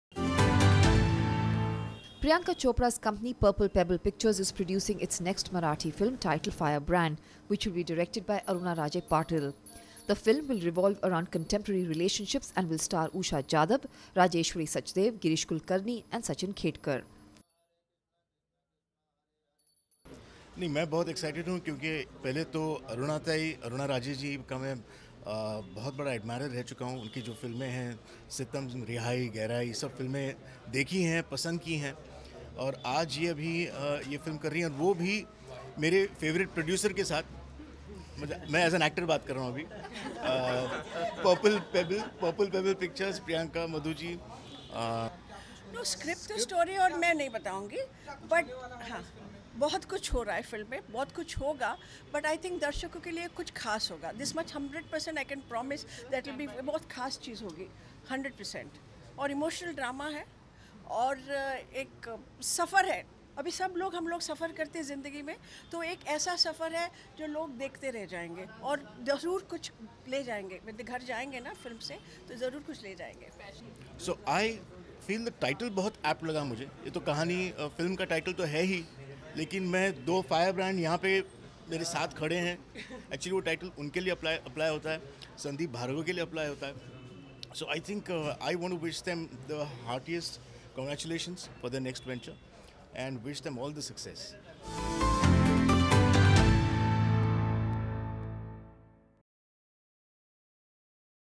Peecee’s Company To Produce Marathi Film Firebrand
Priyanka Chopra’s company Purple Pebble Pictures is producing a third Marathi film titled Firebrand to be directed by Aruna Raje Patil. The film will revolve around contemporary relationships and will  star Usha Jadhav, Rajeshwari Sachdev, Girish Kulkarni and Sachin Khedekar. Purple Pebble Pictures was founded by Chopra and her mother, Madhu Chopra.